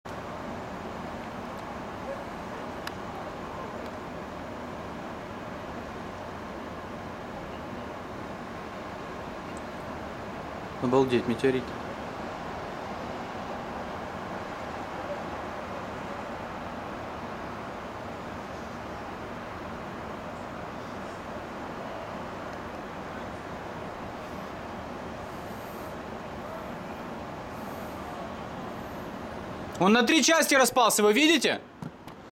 ВИДЕО ДЛЯ ДЕТЕЙ. Метеорит ! Meteorite over Westwood, Los Angeles 07.27.2016 21.42 PM
Кий увидел падение метеорита над Лос Анжелесом !
Meteor Shower !